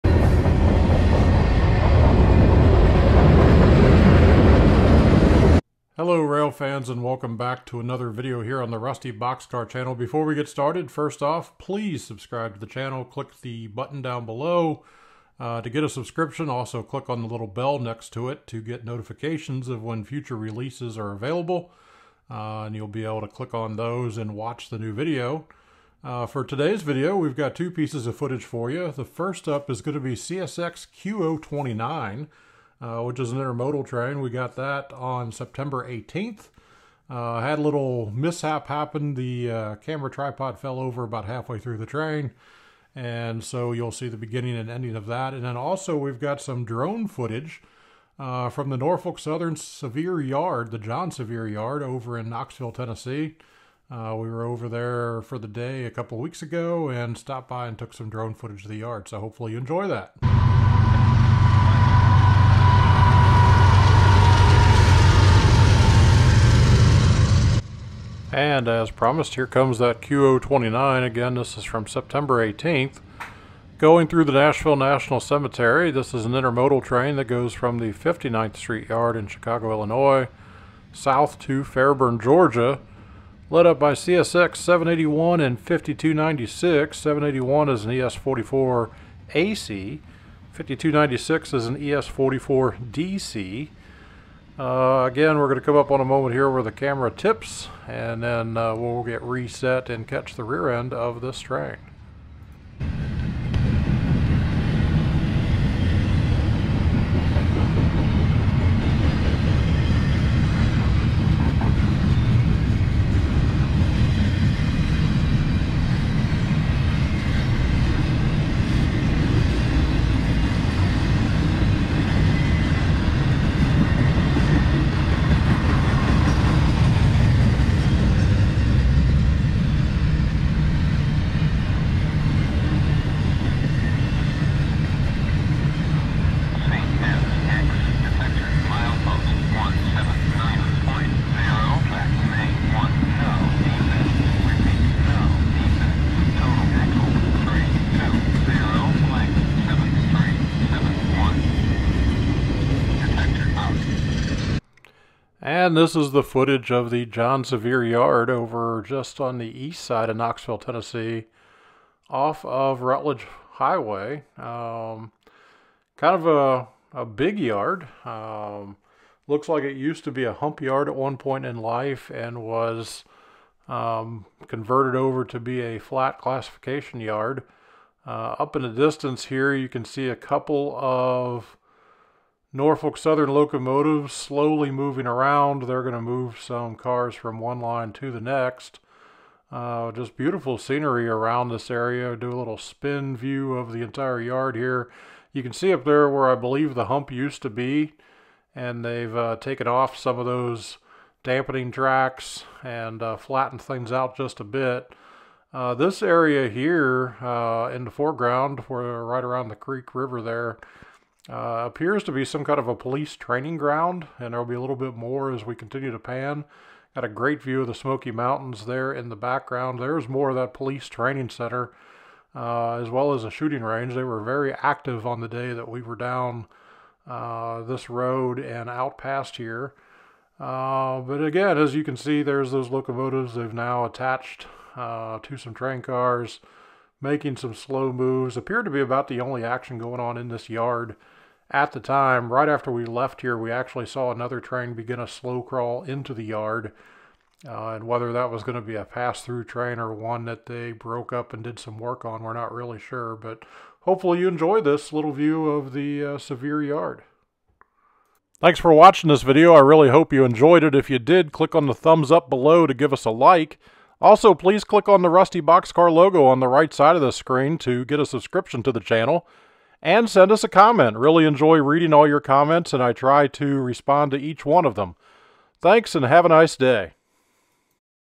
CSX Q029 and Drone Footage of NS John Sevier Yard
Caught an intermodal in Nashville on 9/18, and have some drone footage from Knoxville on 9/10.